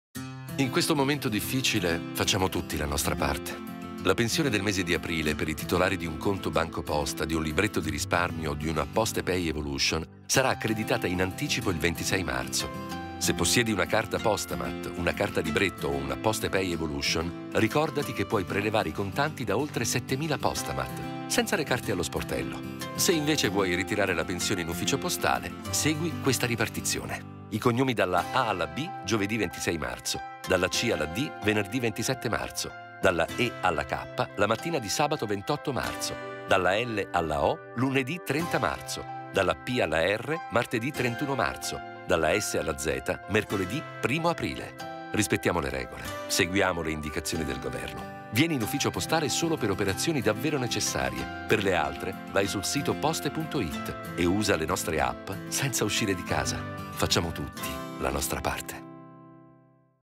Coronavirus, Pensioni di Aprile 2020 - Vademecum di Poste Italiane
Poste Italiane in un video spiega modalità e tempi di erogazione delle pensioni per il mese di Aprile 2020 in emergenza per il contagio da Coronavirus